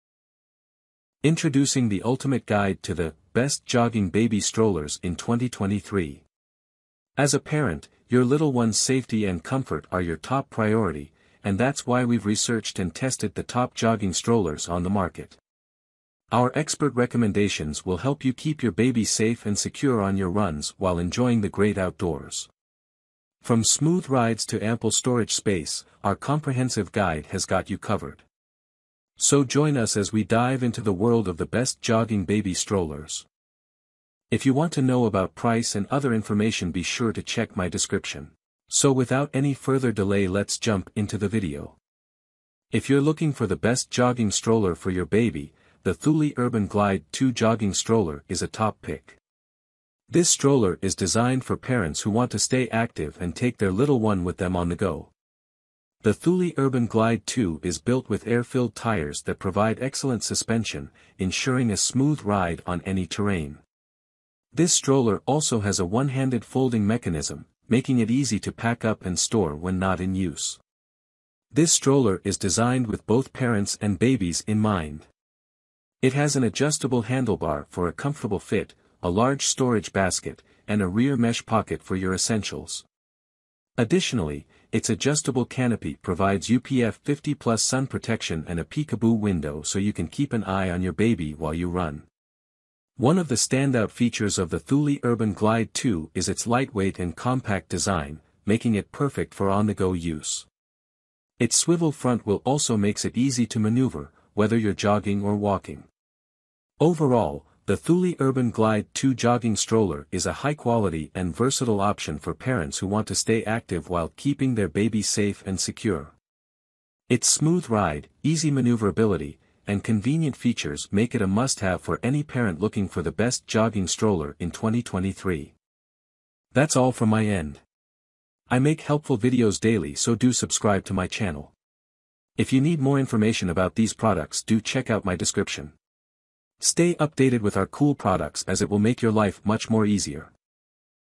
Best Jogging Baby Strollers in 2023: Keep Your Little One Safe and Secure on Your Runs
✔️Best Jogging Baby Strollers 🔥 💥
~ Product Link ~
Looking for the best jogging baby strollers in 2023? In this video, we've rounded up the top jogging strollers that will keep your little one safe and secure while you go for a run.

Our list includes the latest and most innovative jogging strollers, designed to make your runs more comfortable and enjoyable for both you and your baby. We've considered factors such as safety features, ease of use, durability, and overall value for money to bring you the best options in the market.

Whether you're an experienced runner or just starting out, these jogging strollers are guaranteed to meet your needs and exceed your expectations. So, let's dive in and find the perfect jogging stroller for you and your little one!